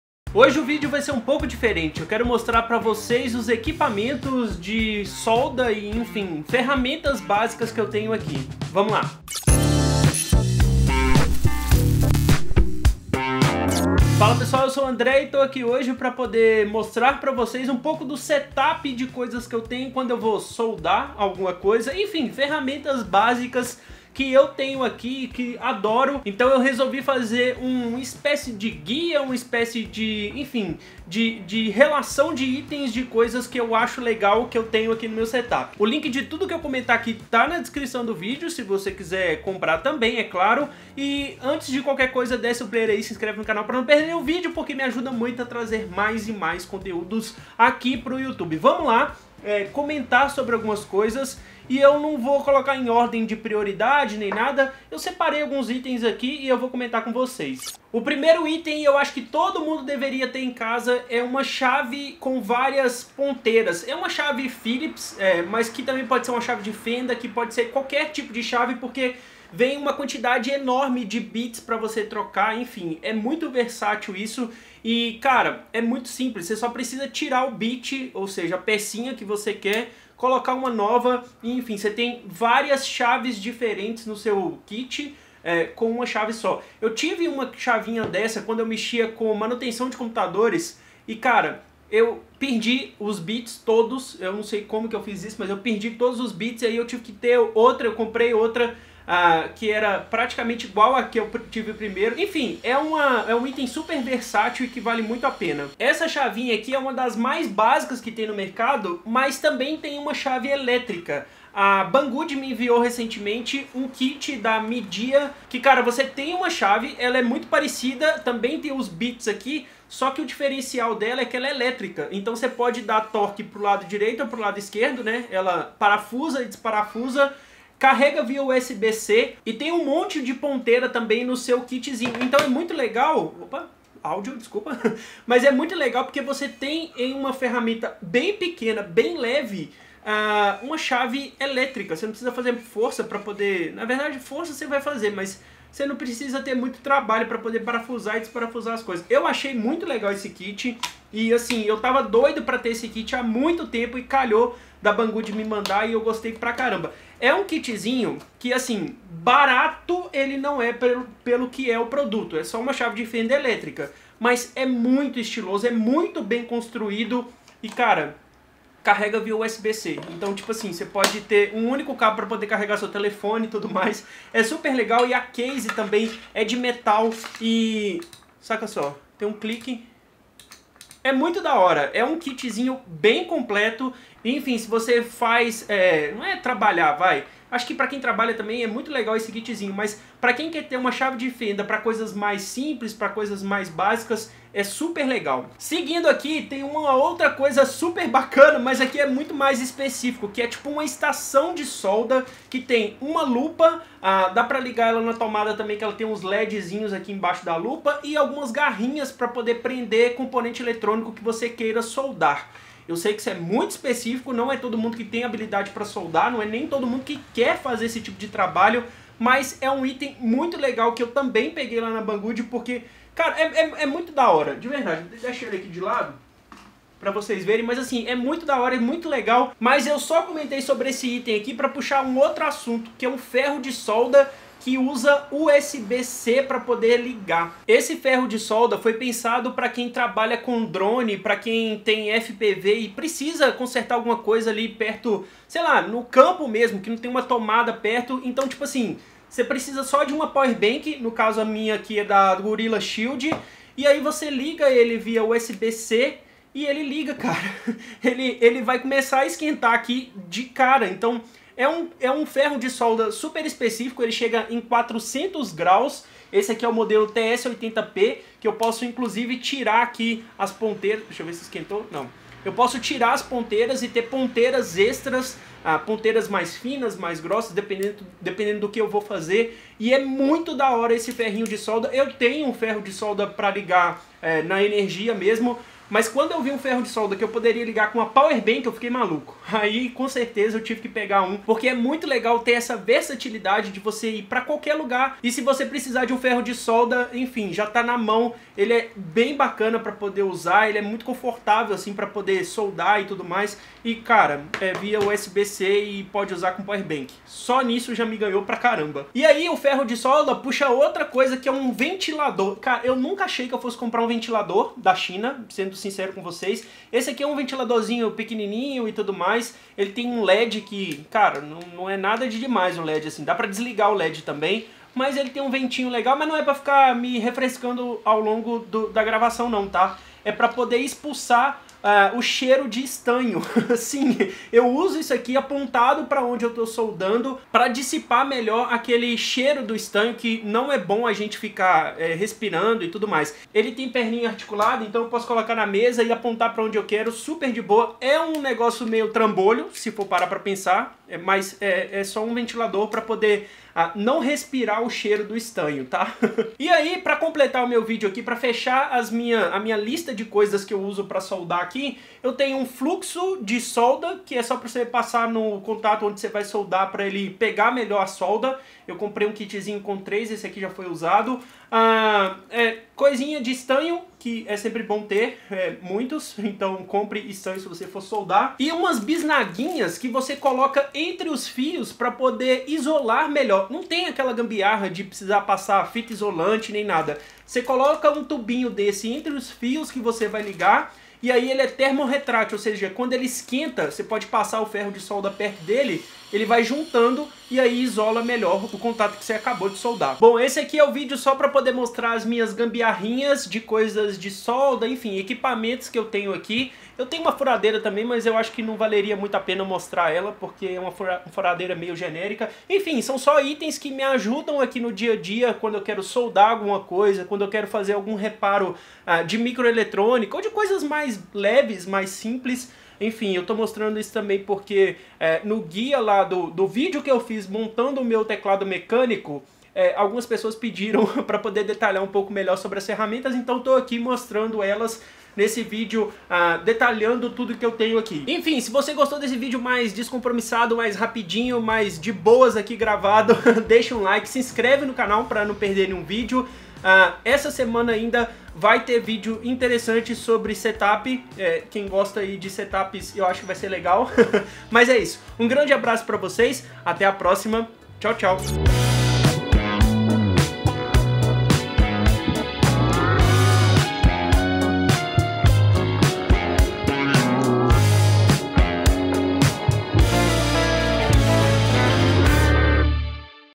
VOCÊ PRECISA DESSAS FERRAMENTAS!!! | FERRO DE SOLDA TS80P E CHAVE ELÉTRICA MIJIA
LINK PRA TODOS OS PRODUTOS NA DESCRIÇÃO:

Fiz uma seleção de ferramentas que tenho aqui no meu pequeno laboratório e esse vídeo é pra mostrar tudo em detalhes!

PRODUTOS
Chave com muitos Bits